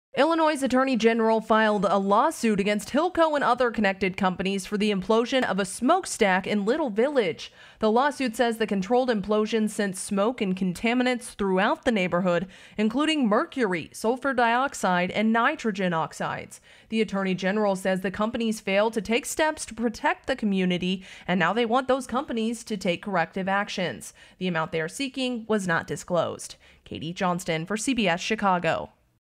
Illinois AG Files Suit Against Hilco After Implosion Dust Covers Little Village
A month after the implosion of a smokestack in Little Village left a neighborhood covered in a cloud of dust, Illinois Attorney General Kwame Raoul filed suit against Hilco and other connected companies for the blast on April 11. Katie Johnston reports.